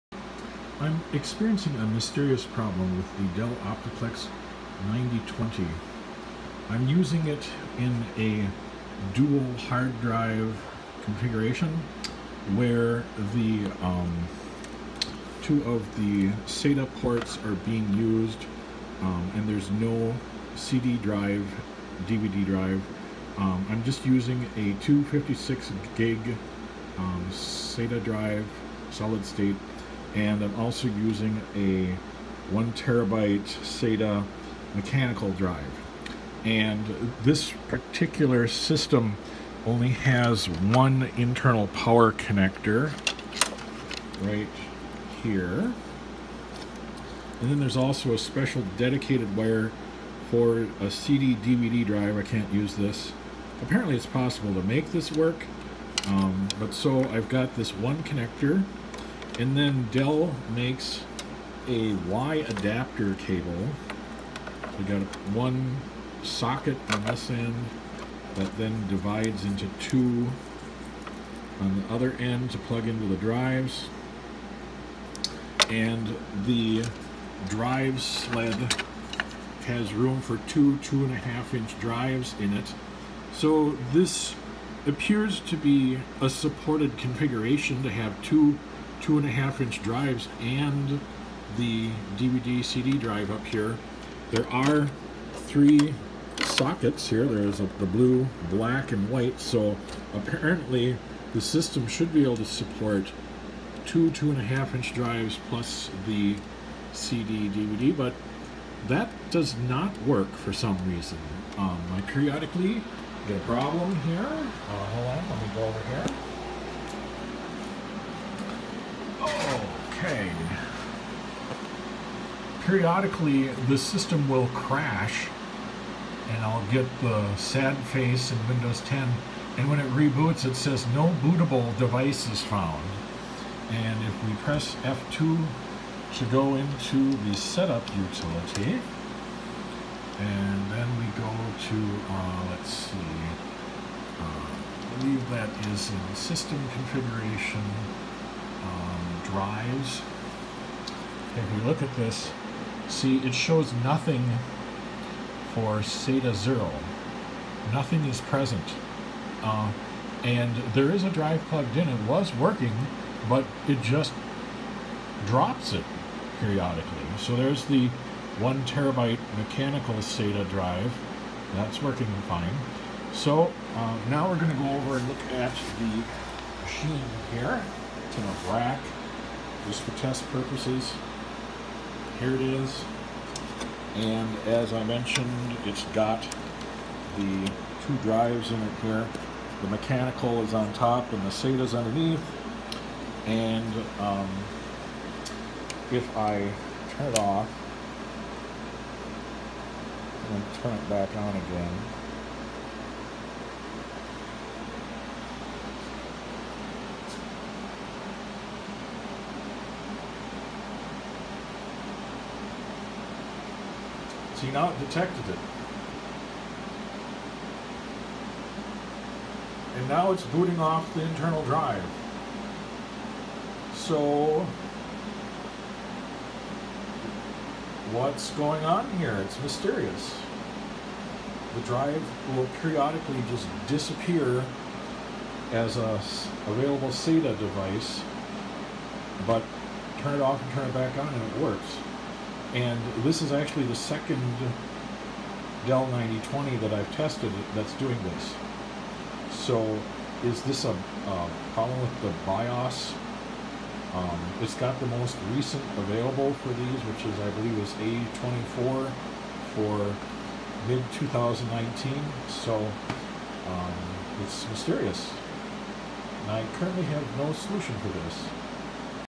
Dell 9020, 2 HD, Boot drive disappears
I’m having a problem using a Dell OptiPlex 9020 with two hard drives. The boot drive is a 256 gig SSD, while the second drive is a 1 TB mechanical.

After being powered on and running for some amount of time, usually less than a day, the boot drive will suddenly disconnect. Windows 10 becomes unresponsive because it can’t access the drive and crashes.
After rebooting the system stops at a boot error that says “No bootable devices found.”

If I turn off the computer via the power button and then turn it back on, it now suddenly detects the boot drive without me having done any hardware changes, plugging or unplugging power or data cables, and windows 10 boots properly.

Is this some sort of BIOS firmware defect? I’m using version A24 which is the most current for summer 2019 for this system.

Possibly it is some sort of power overload problem from having the two drives plugged into one power cable? But that seems to be a standard configuration for this system.